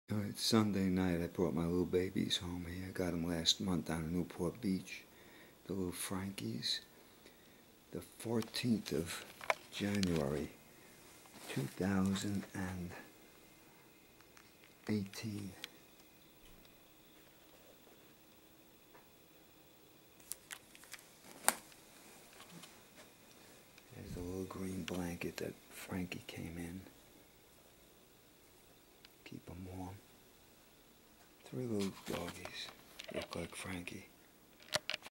Joe Nania 3 DOGS January 14 2018 Joe Nania
Here I am JOE NANIA on Sunday night at 7:46 p.m. January 14 2018 with my 3 little white dogs - yes 3 cute  little white stuffed animal dogs that I bought on Newport Beach -